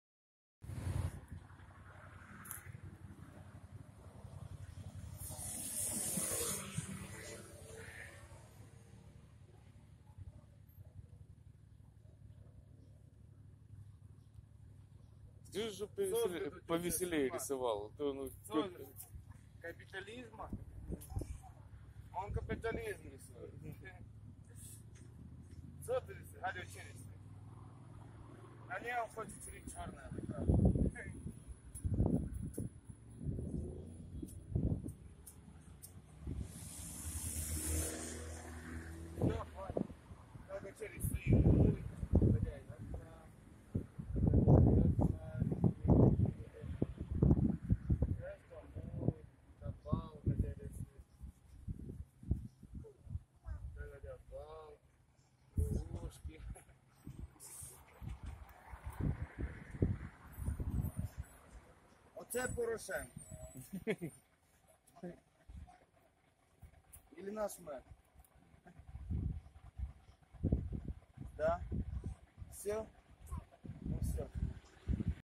АККЕРМАН !!! УДАРИМ КРАСКОЙ ПО КАПИТАЛИЗМУ !!!
1 Мая 2018 года 10.00  АККЕРМАН !!! УДАРИМ КРАСКОЙ ПО КАПИТАЛИЗМУ !!! У каждого дома осталось немного краски после прошлогоднего ремонта, которая совсем засохнет если ее не использовать. Плесни ее в лицо капитализму! Сообща напишем картину маслом в виде безжалостной борьбы с КАПИТАЛИЗМОМ. Этим мы покажем всему миру, что власть денежного мешка заканчивается в Аккермане! Или кому то нравится существовать в матрице необходимости? Концепция проекта : "Мы за жизнь! Мы против - измов" За каждым измом стоит идеология, которая говорит: Кто не с нами, тот против нас, и значит нужно их убить. Война всех против всех, абсолютный хаос и исчезновение планеты Земля со стратегической карты галактики. Жить и радоваться жизни самой куда лучше.    «Вся власть воображению!»
Это один из лозунгов «парижской весны».